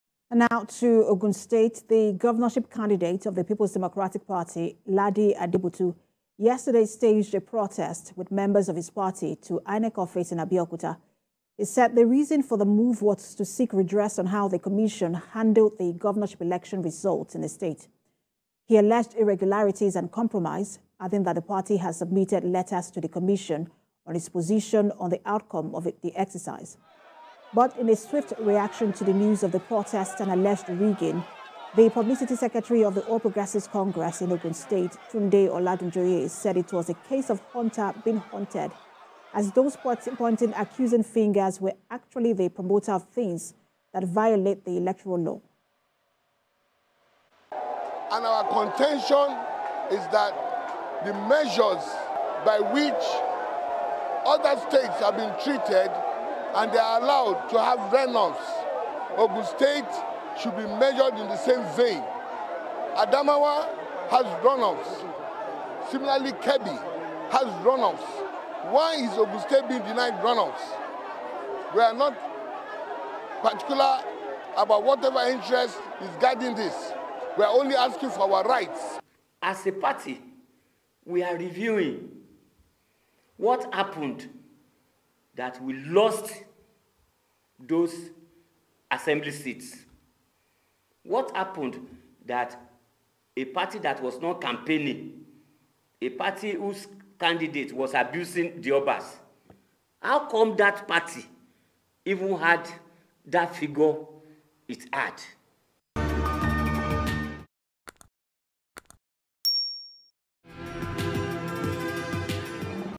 PDP Candidate Adebutu Leads Protest To INEC Office
In Ogun state, the Governorship Candidate of the Peoples Democratic Party, Ladi Adebutu yesterday staged a protest with members of his party to INEC office in Abeokuta.
He said the reason for the move was to seek redress on how the commission handled the Governorship Election result in the state.
He alleged irregularities and compromise, adding that the party has submitted letters to the Commission on its position on the outcome of the exercise.